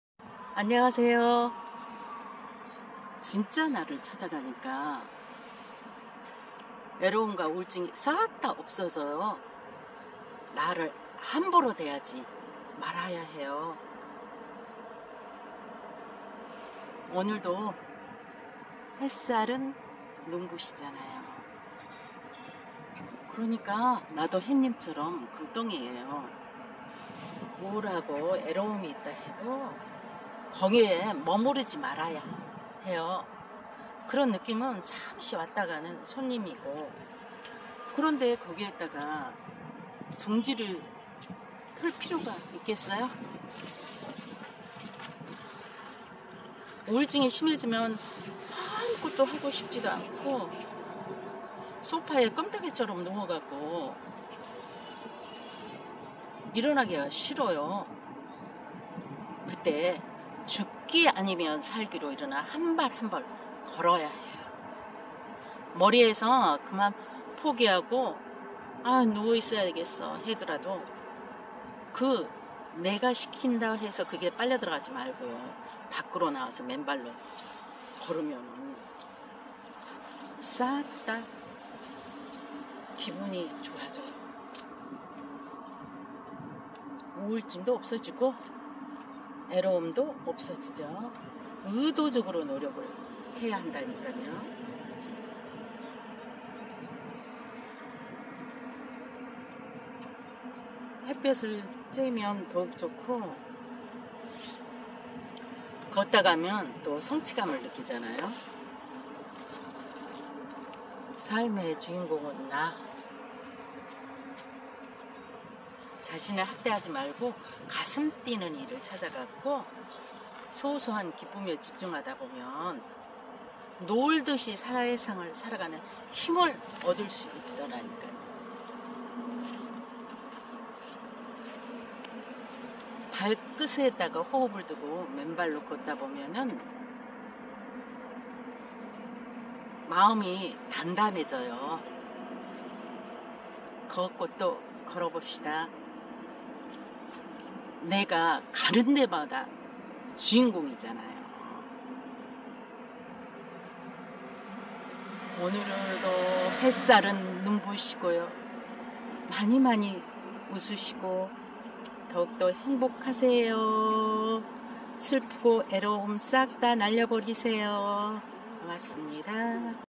나를 함부로 대하지 말아요.진짜 나를 찾아가니까 외로움과 우울증이 싹~다 없어져요. 걷고 또 걸어보자.가는데마다 내가 주인공이다.." 오늘도 하늘은 푸르다. "
나를 함부로 대하지 말아요.
진짜 나를 찾아가니까 외로움과 우울증이
싹~다 없어져요.
오늘도 햇살이 눈부시네요.
나도 햇님같은 금덩어리예요.
우울하고 외로움이 왔다해도 거기에 머무르지 않아야 한다.
슬픈 느낌도 잠시 왔다가는 손님이니
거기에 둥지를 틀지 말아야 한다.
우울증이 심해지면 아무것도 하고 싶지않고
소파에 껌딱지처럼 누워 있기만 한다.
죽기 아니면 살기로 일어나 한발 한발 걸어라.
머리에서 그만 포기하고 누워있자해도
밖으로 나와 이왕이면 맨발로 걷는다.
의도적으로 노력해야한다.
걷다보면 성취감을 느낄 수 있다.
삶의 주인공은 나다.
자신을 학대하지말고 가슴 뛰는 일을 찾아라.
소소한 기쁨에 집중하다보면
놀듯이 세상을 살아가는 힘을 얻을 수 있다.
마음이 단단해지게 발끝으로 호흡하면서
걷고 또 걸어보자.
가는데마다 내가 주인공이다..
" 오늘도 하늘은 푸르다. "